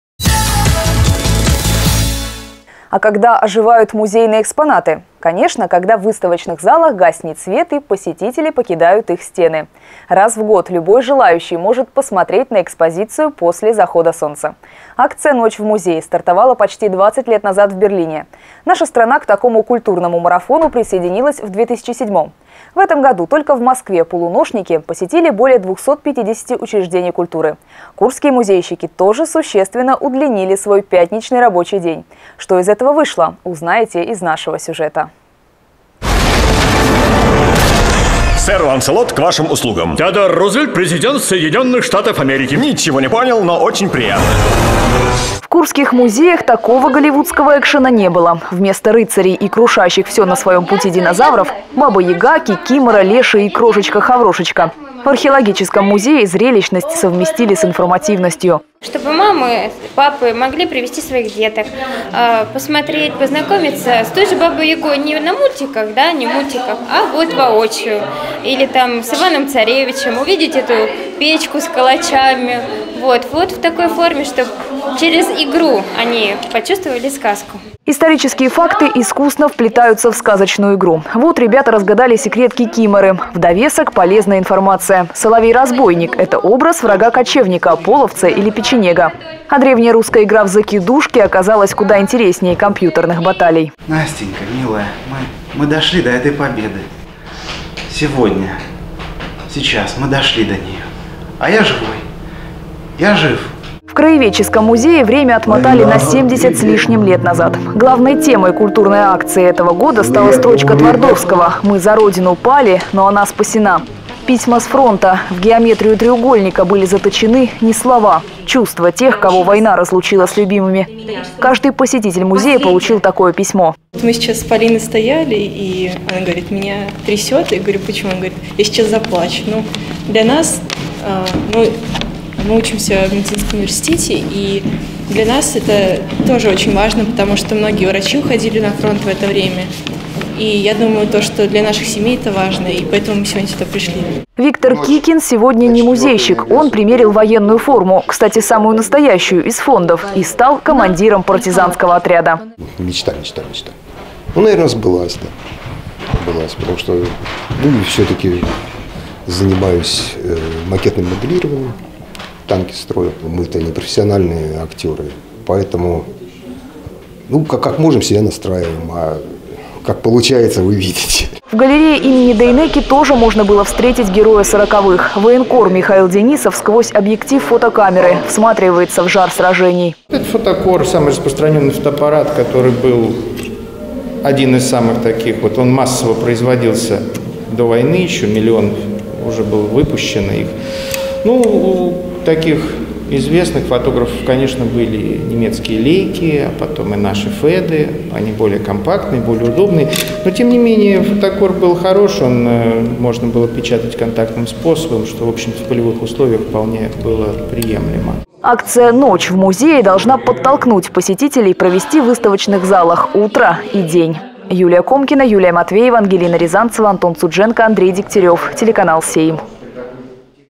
Куряне ходили ночью по музеям
Когда оживают музейные экспонаты? Конечно, когда в выставочных залах гаснет свет и посетители покидают их стены. Раз в год любой желающий может посмотреть на экспозицию после захода солнца. Акция «Ночь в музее» стартовала почти 20 лет назад в Берлине. Наша страна к такому культурному марафону присоединилась в 2007. В этом году только в Москве полуночники посетили более 250 учреждений культуры. Курские музейщики тоже существенно удлинили свой пятничный рабочий день.